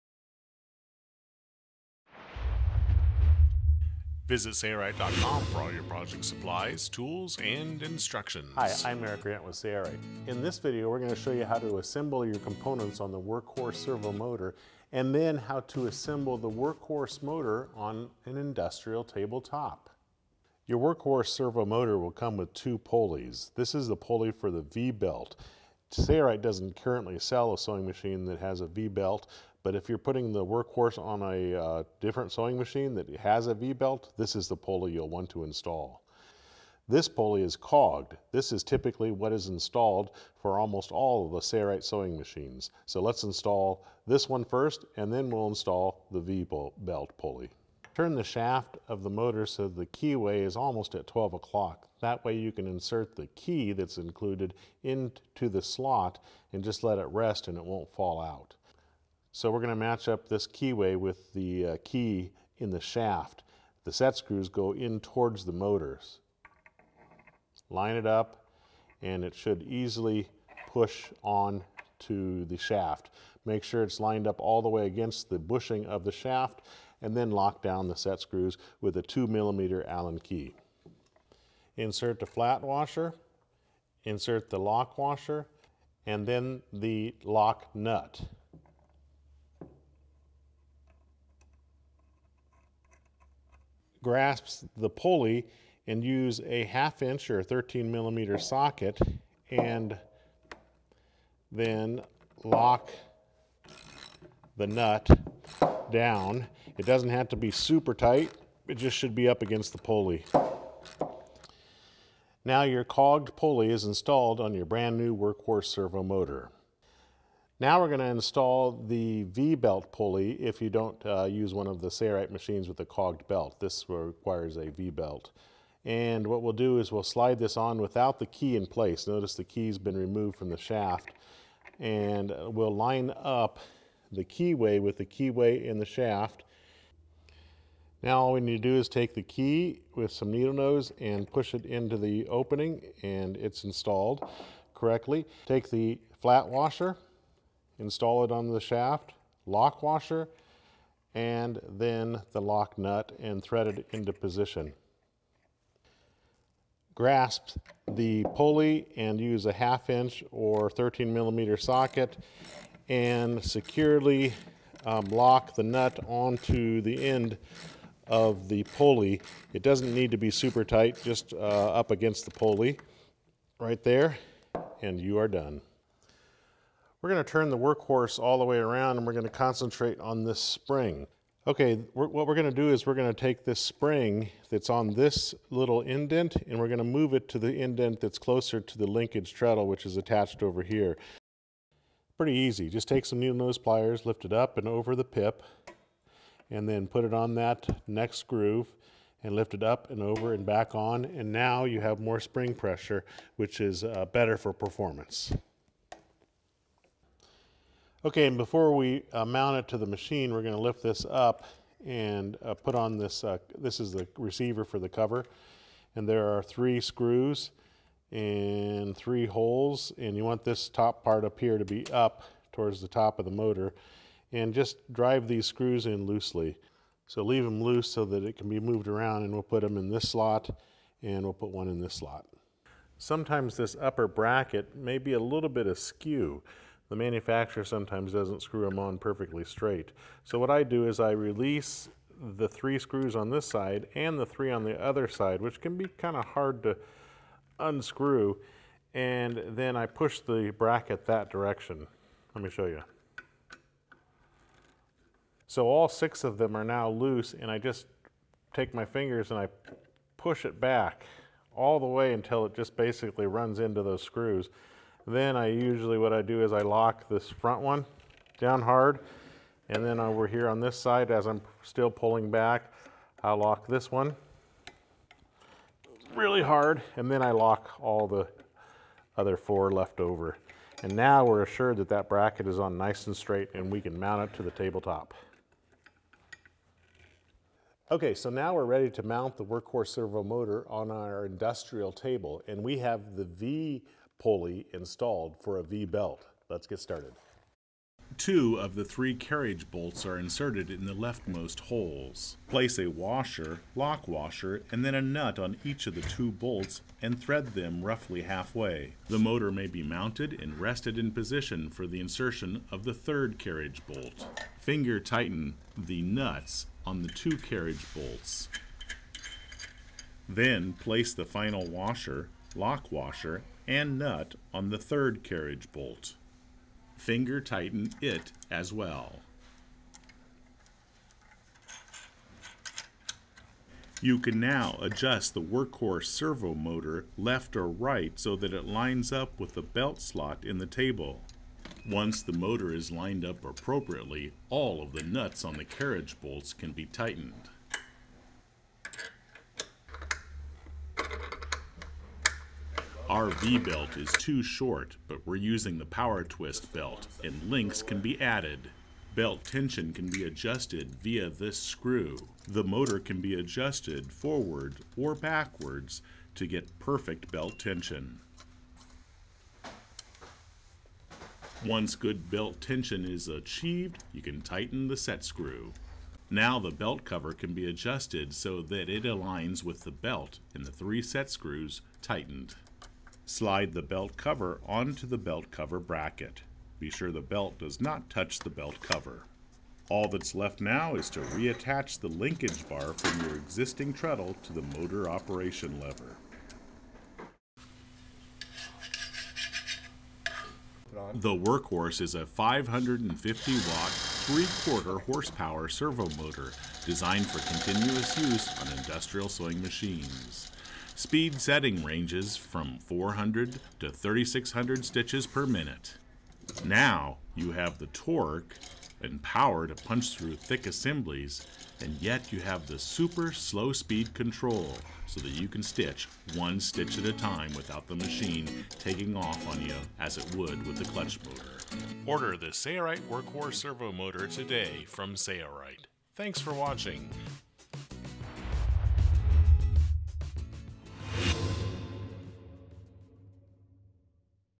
Installing Workhorse Servo Motor on Industrial Table
This video shows how to install the Workhorse Servo Motor to an Industrial Sewing Machine Table. It is easy to replace your clutch motor with the Sailrite Workhorse Servo Motor for slow speed control and optimal power.